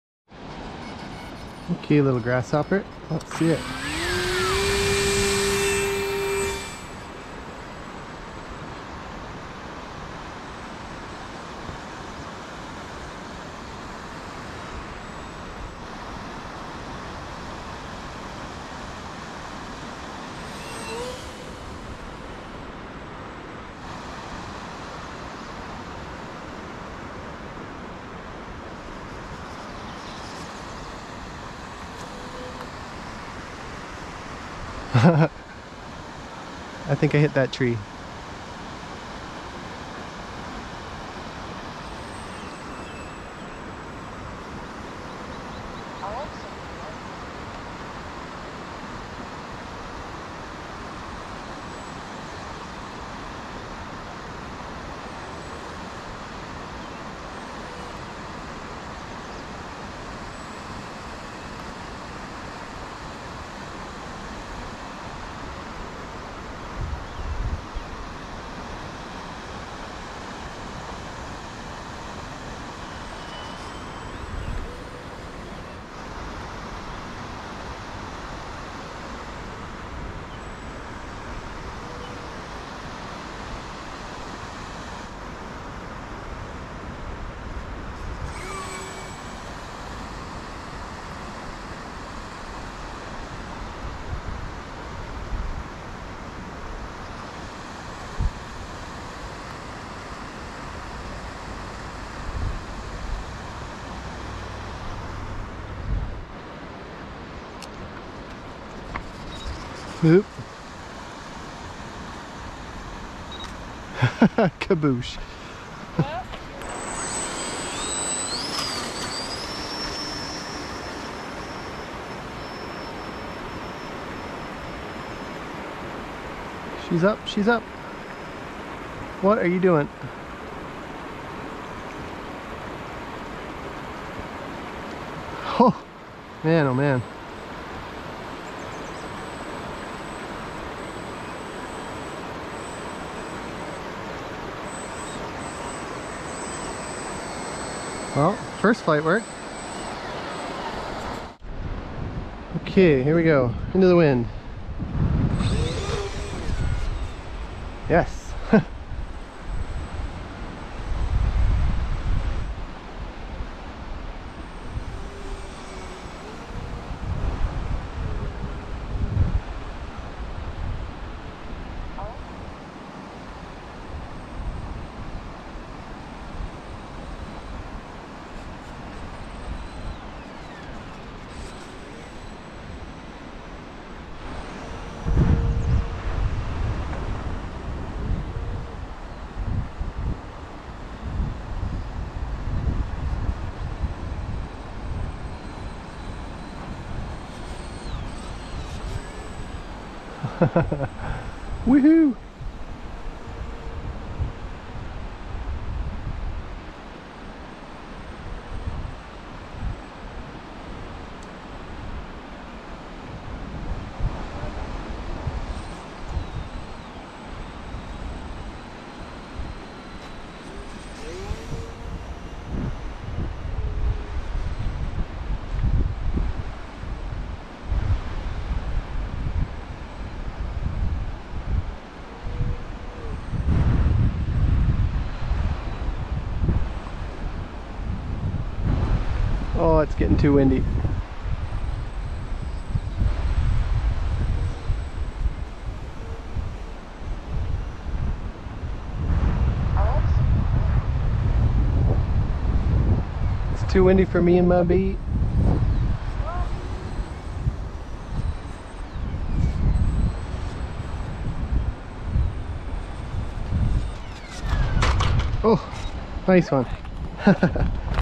Squamish 2024 06 09 - Wild Flights
Flying the UMX Conscendo and Waco with a wilderness backdrop.